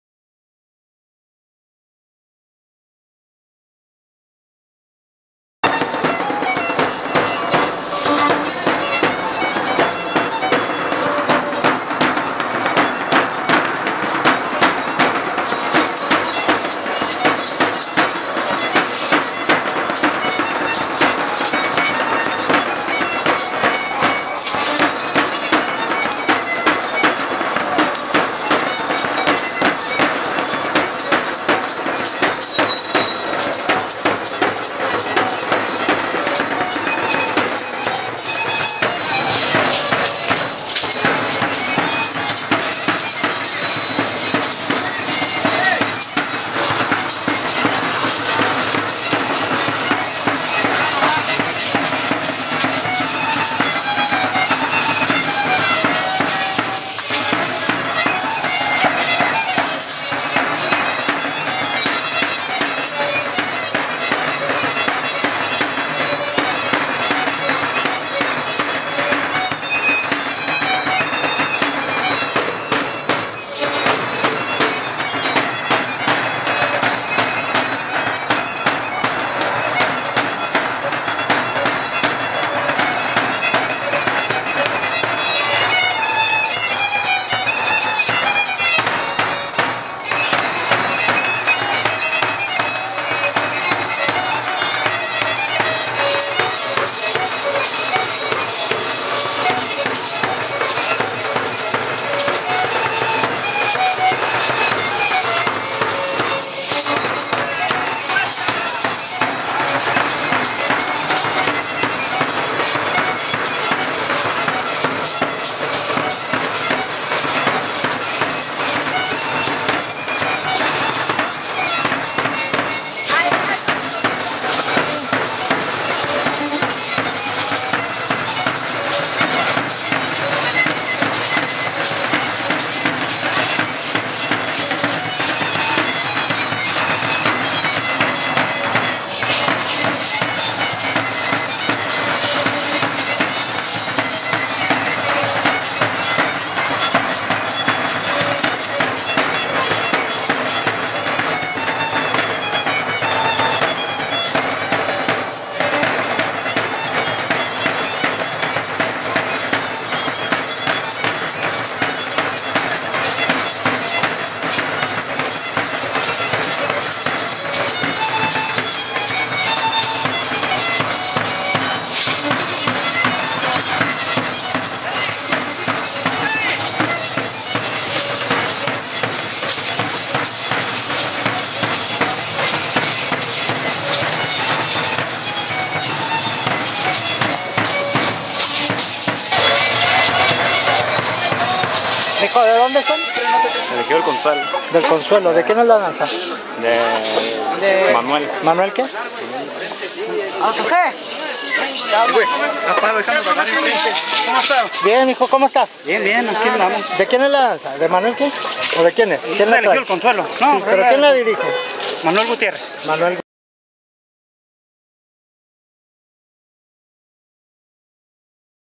Danza de Pluma del Consuelo, Coah
En las Fiestas Patronales de San Lorenzo en el Ejido La Esperanza, Mpio. de Matamoros, Coah. el 8 de agosto de 2011